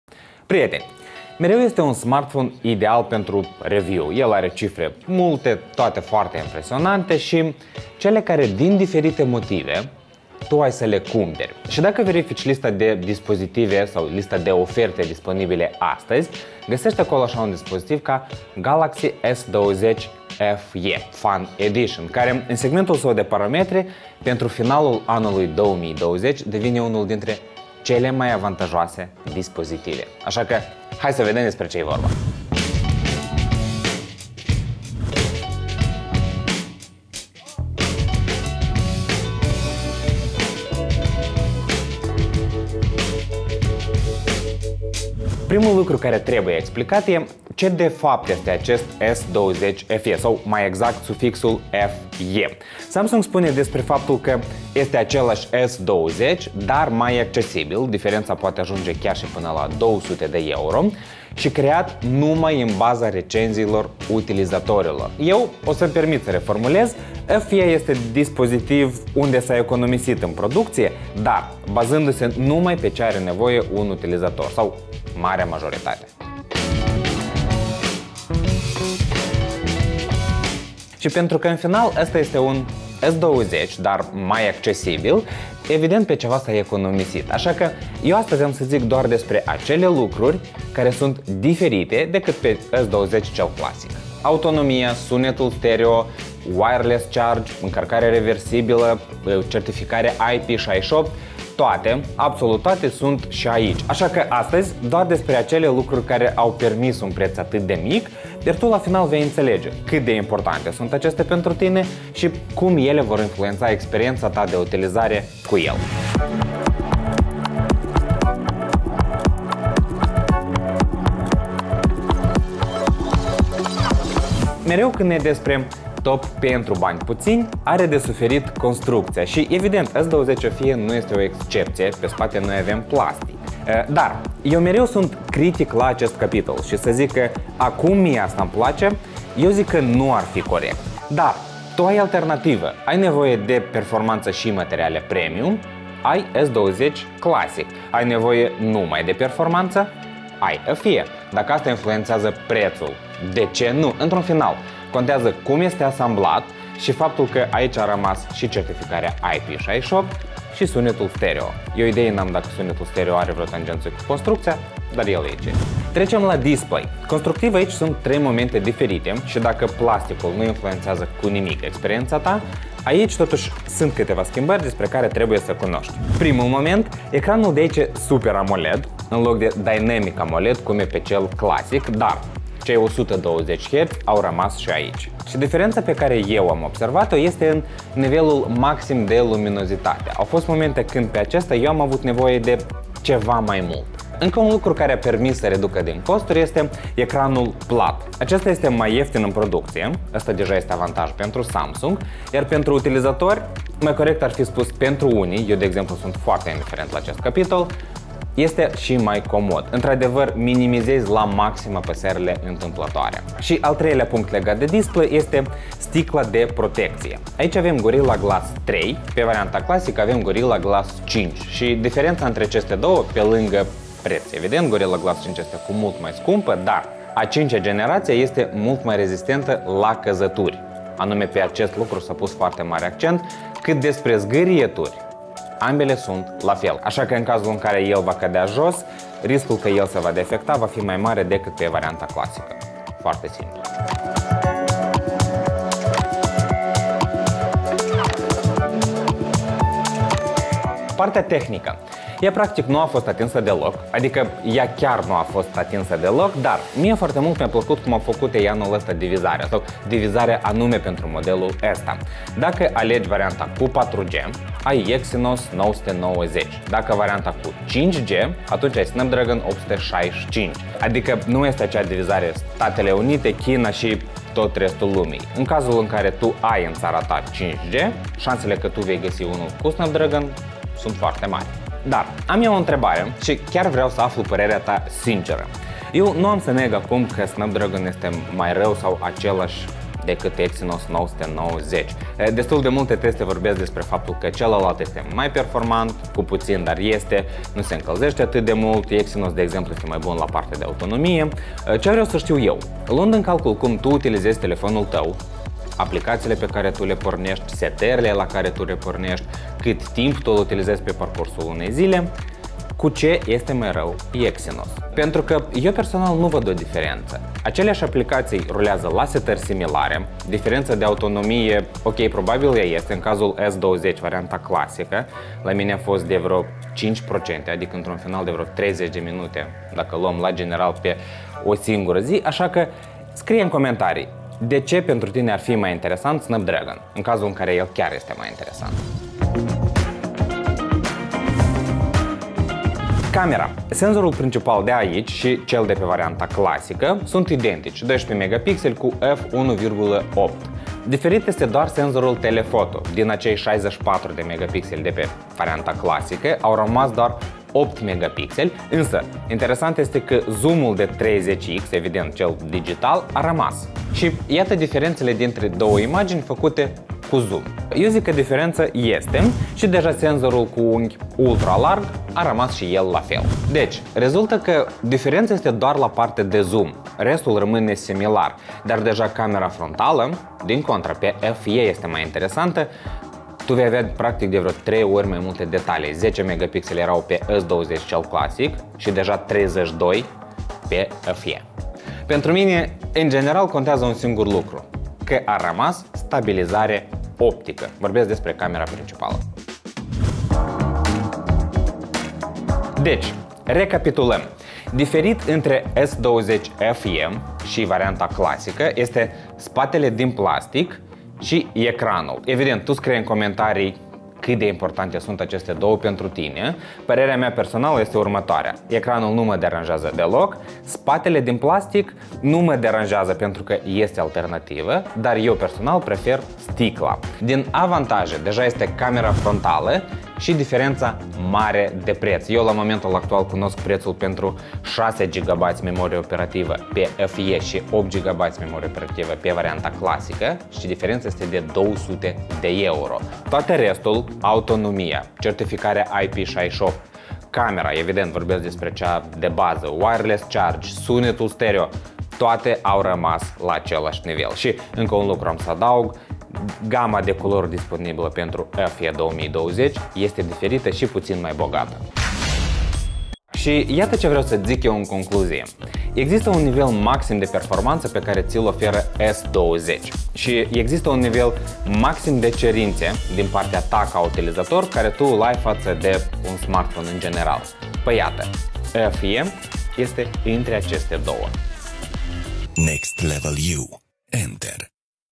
Samsung Galaxy S20 FE: S-a economisit pe ... (review română)
Mereu este un smartphone ideal pt review, cu cifre ”multe și impresionante”. Și cele care din diferite motive tu ai să-l cumperi.
Și dacă verificăm lista de oferte disponibilă azi, găsești acolo S20 FE, care în segmentul său de ”parametri” devine cel mai avantajos pentru finele lui 2020.